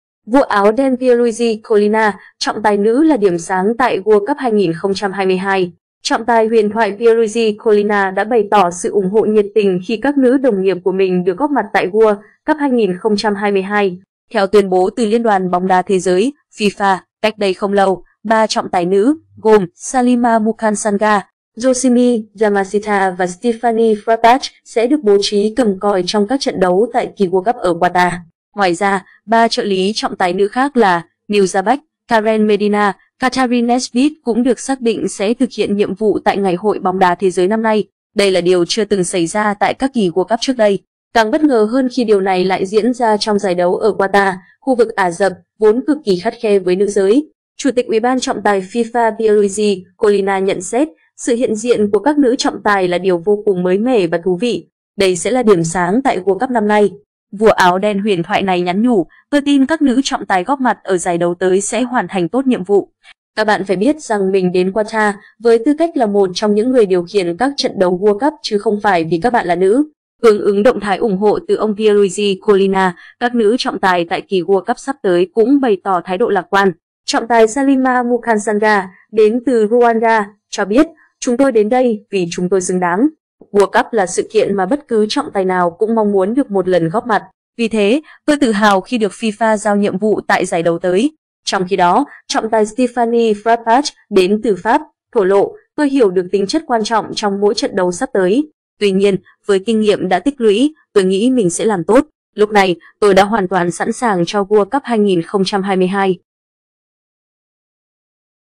'Vua áo đen' Pierluigi Collina: Trọng tài nữ là điểm sáng tại World Cup 2022
Trọng tài huyền thoại Pierluigi Collina đã bày tỏ sự ủng hộ nhiệt tình khi các nữ đồng nghiệp của mình được góp mặt tại World Cup 2022.3 nữ trọng tài Mukansanga, Yamashita và Frappart góp mặt tại World Cup 2022 (Ảnh: Sport Bible)
Trọng tài huyền thoại Collina bày tỏ sự ủng hộ các nữ trọng tài cầm còi tại World Cup (Ảnh: Daily Mail)
Theo tuyên bố từ LĐBĐ Thế giới (FIFA) cách đây không lâu, 3 trọng tài nữ - gồm: Salima Mukansanga, Yoshimi Yamashita và Stephanie Frappart - sẽ được bố trí cầm còi trong các trận đấu tại kỳ World Cup ở Qatar.
"Vua áo đen" huyền thoại này nhắn nhủ: "Tôi tin các nữ trọng tài góp mặt ở giải đấu tới sẽ hoàn thành tốt nhiệm vụ. Các bạn phải biết rằng mình đến Qatar với tư cách là 1 trong những người điều khiển các trận đấu World Cup chứ không phải vì các bạn là nữ".
Chủ tịch Ủy ban Trọng tài FIFA Pierluigi Collina nhận xét: "Sự hiện diện của các nữ trọng tài là điều vô cùng mới mẻ và thú vị. Đây sẽ là điểm sáng tại World Cup năm nay".
Ngoài ra, 3 trợ lý trọng tài nữ khác là: Neuza Back, Karen Medina, Kathryn Nesbitt cũng được xác định sẽ thực hiện nhiệm vụ tại ngày hội bóng đá thế giới năm nay.
Trọng tài Salima Mukansanga, đến từ Rwanda, cho biết: "Chúng tôi đến đây vì chúng tôi xứng đáng. World Cup là sự kiện mà bất cứ trọng tài nào cũng mong muốn được một lần góp mặt. Vì thế, tôi tự hào khi được FIFA giao nhiệm vụ tại giải đấu tới".
Nữ trọng tài người Rwanda tự tin thực hiện nhiệm vụ tại World Cup 2022 (Ảnh: Goal)
Đây là điều chưa từng xảy ra tại các kỳ World Cup trước đây. Càng bất ngờ hơn khi điều này lại diễn ra trong giải đấu ở Qatar, khu vực Ả rập - vốn cực kỳ khắt khe với nữ giới.
Trong khi đó, trọng tài Stephanie Frappart, đến từ Pháp, thổ lộ: "Tôi hiểu được tính chất quan trọng trong mỗi trận đấu sắp tới. Tuy nhiên, với kinh nghiệm đã tích lũy, tôi nghĩ mình sẽ làm tốt. Lúc này, tôi đã hoàn toàn sẵn sàng cho World Cup 2022".
Hướng ứng động thái ủng hộ từ ông Pierluigi Collina, các nữ trọng tài tại kỳ World Cup sắp tới cũng bày tỏ thái độ lạc quan.